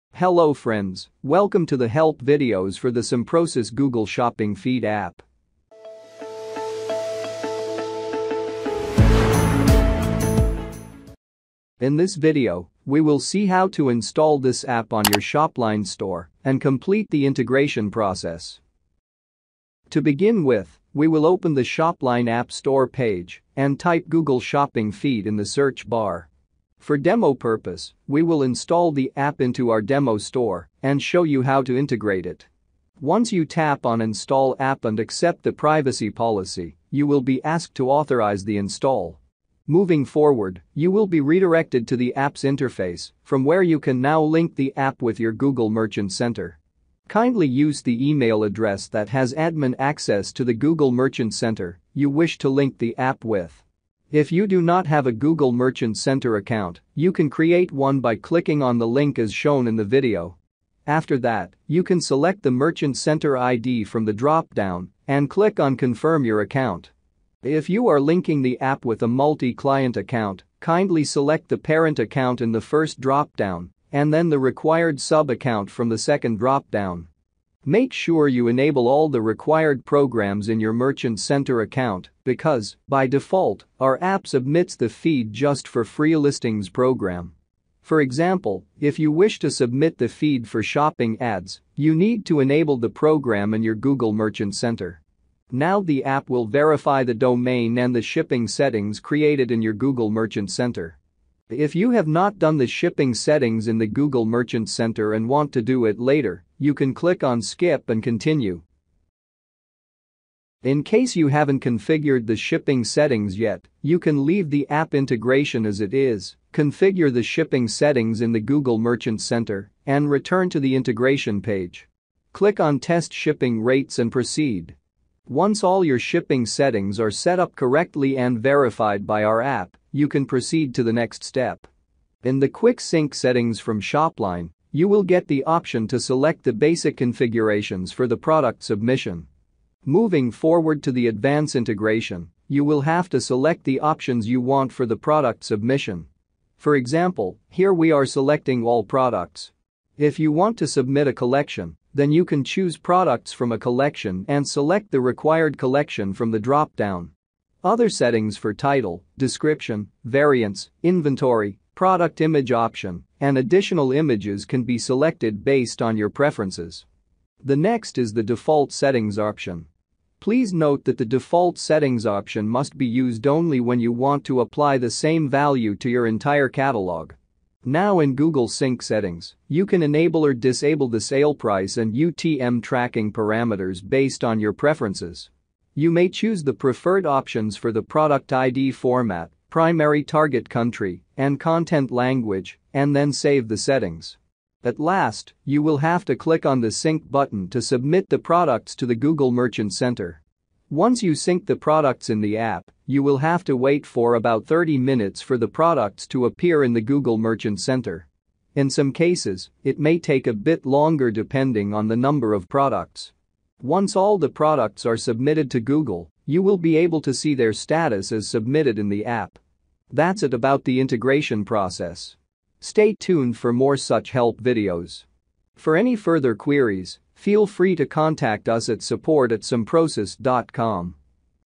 Step by Step Integration of the Google Shopping Feed App for Shopline
Refer to this help video for the entire process in detail on a step-by-step installation Guide of the Shopping feed App for Shopline

What You Will Get in This Video:

00:00 Introduction
00:13 Install this app in your Shopline store
00:42 Process in our app

Shopline, Simprosys, Google Merchant Center, Shopping Feed, Integration, Shopping Ads